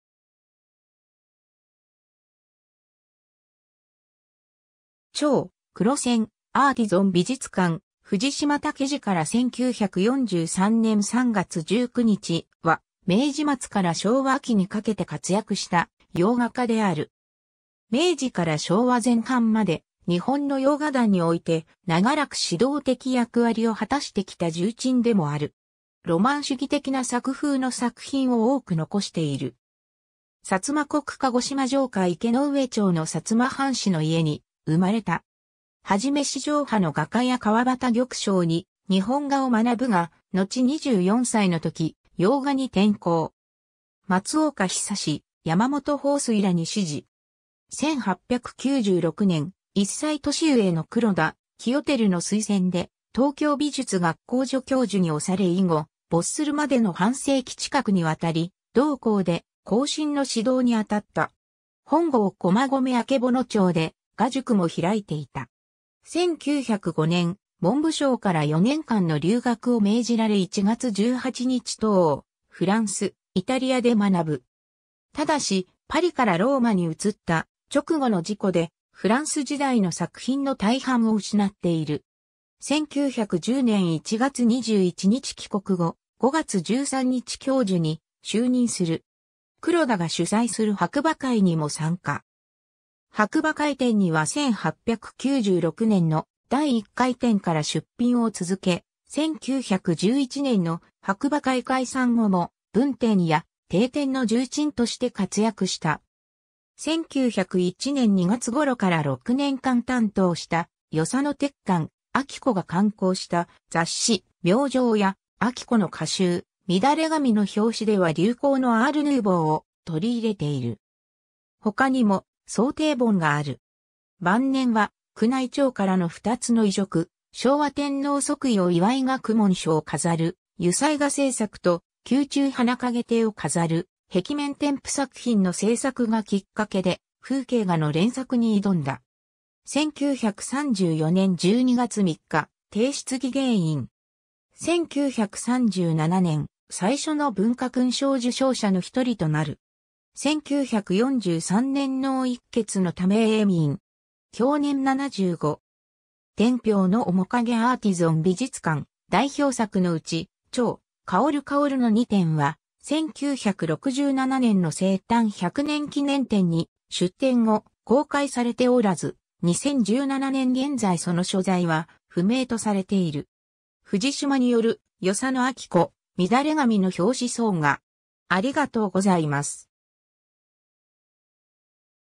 藤島武二
『蝶』 （1904年） 『黒扇』 アーティゾン美術館（1908年 - 1909年） 藤島 武二（ふじしま たけじ、1867年10月15日（慶応3年9月18日） - 1943年（昭和18年）3月19日）は、明治末から昭和期にかけて活躍した洋画家である。
明治から昭和前半まで、日本の洋画壇において長らく指導的役割を果たしてきた重鎮でもある。
ロマン主義的な作風の作品を多く残している。
 薩摩国鹿児島城下池之上町（現在の鹿児島市池之上町）の薩摩藩士の家に生まれた。
 はじめ四条派の画家や川端玉章に日本画を学ぶが、のち24歳の時、洋画に転向（日本画の作品は殆ど現存しない）。
松岡寿・山本芳翠らに師事。
1896年（明治29年）、1歳年上の黒田清輝の推薦で東京美術学校（現・東京藝術大学）助教授に推され以後、没するまでの半世紀近くにわたり同校で後進の指導にあたった。
本郷駒込曙町（現・本駒込1丁目）で画塾も開いていた。
1905年（明治38年）、文部省から4年間の留学を命じられ1月18日渡欧、フランス、イタリアで学ぶ。
ただし、パリからローマに移った直後の事故で、フランス時代の作品の大半を失っている。
1910年1月21日帰国後、5月13日教授に就任する。
 黒田が主宰する白馬会にも参加。
白馬会展には1896年（明治29年）の第1回展から出品を続け、1911年（明治44年）の白馬会解散後も文展や帝展の重鎮として活躍した。
 1901年（明治34年）2月ごろから6年間担当した与謝野鉄幹・晶子が刊行した雑誌「明星」や、晶子の歌集『みだれ髪』の表紙では流行のアール・ヌーヴォーを取り入れている。
ほかにも装丁本がある。
 晩年は宮内庁からの2つの依嘱、昭和天皇即位を祝い学問所を飾る油彩画制作と、宮中花蔭亭を飾る壁面添付作品の制作が切っ掛けで風景画の連作に挑んだ。
1934年（昭和9年）12月3日帝室技芸員。
1937年（昭和12年）、最初の文化勲章受章者の一人となる。
1943年脳溢血のため永眠。
享年75。
 『天平の面影』 アーティゾン美術館（1902年） ※代表作のうち、『蝶』、『芳蕙』の2点は、1967年の生誕100年記念展（ブリヂストン美術館）に出展後、公開されておらず、2017年現在その所在は不明とされている。
 藤島による与謝野晶子『みだれ髪』の表紙装画（1901年）